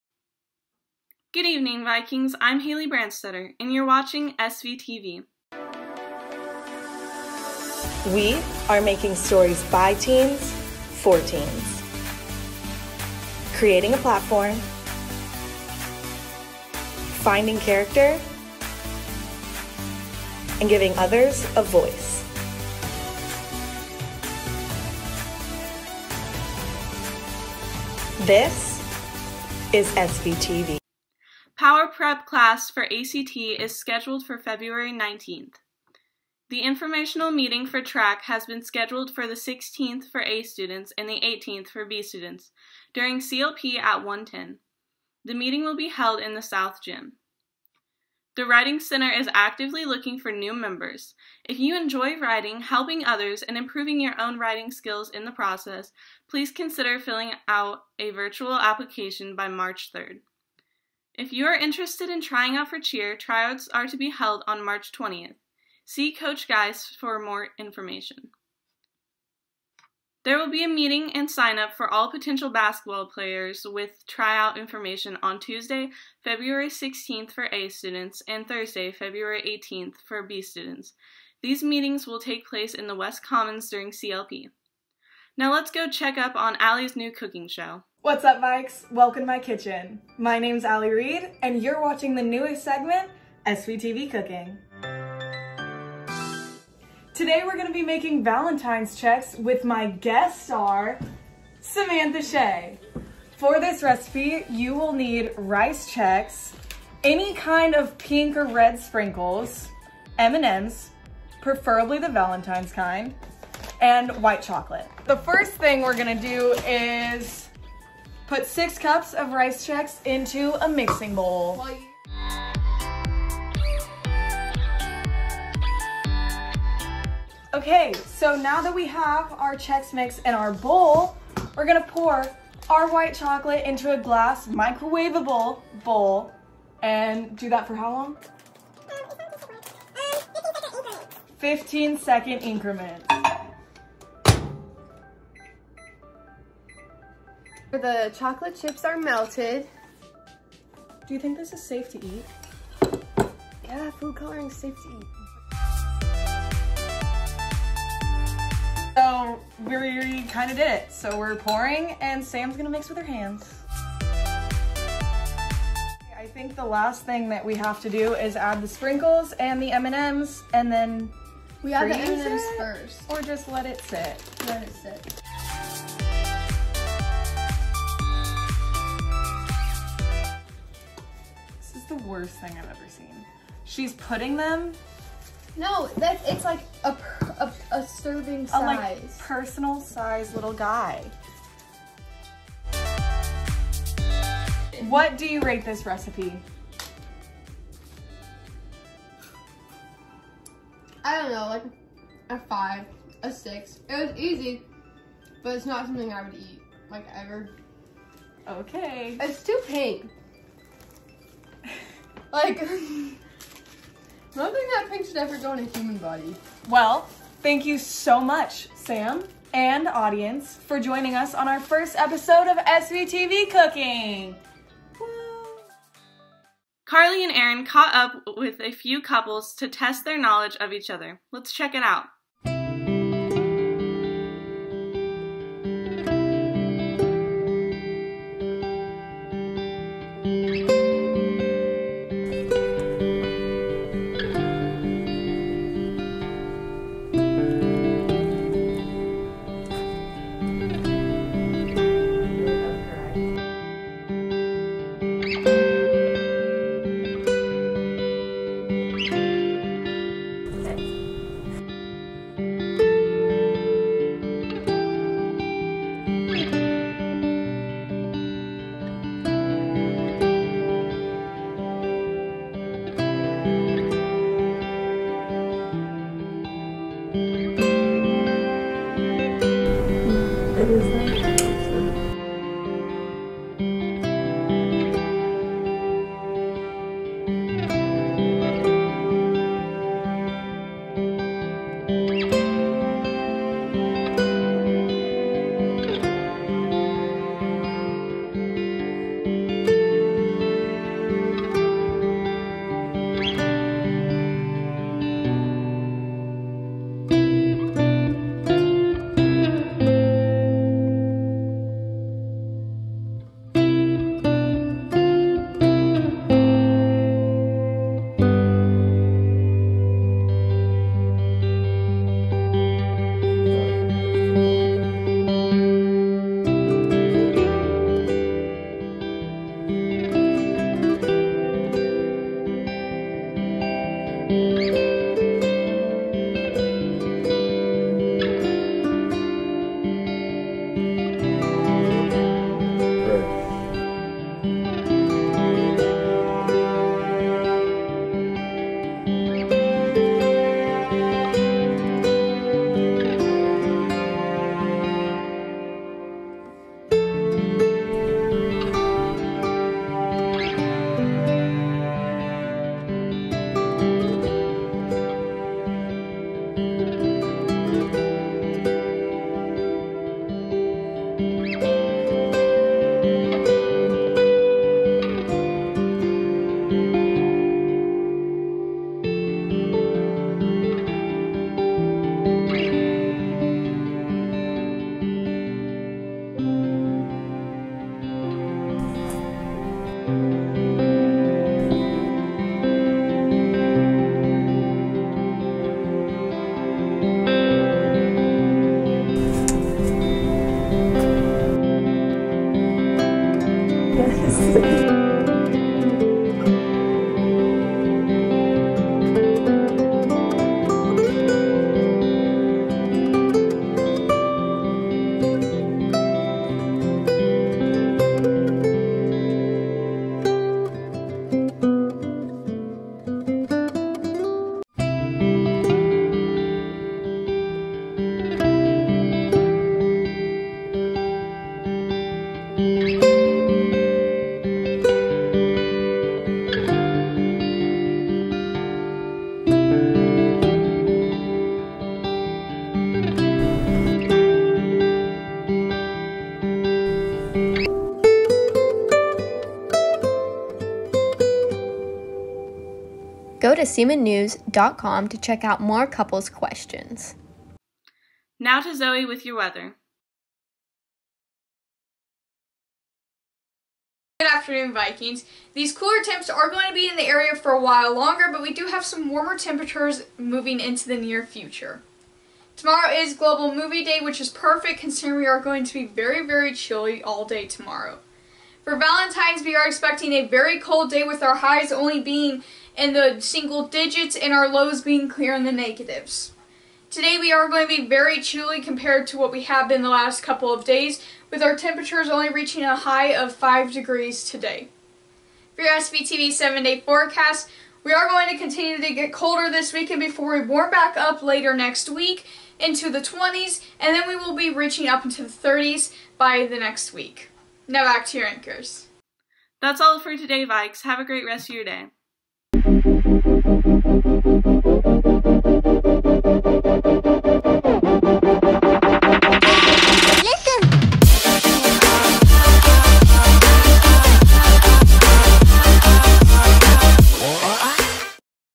2-12-2021 SVTV Friday Report
Stories: Carly Bond and Erin Feldkamp; Ali Reed
Anchors: Haley Branstetter
Weather: Zoey Terrel
Editor: Landon Stuke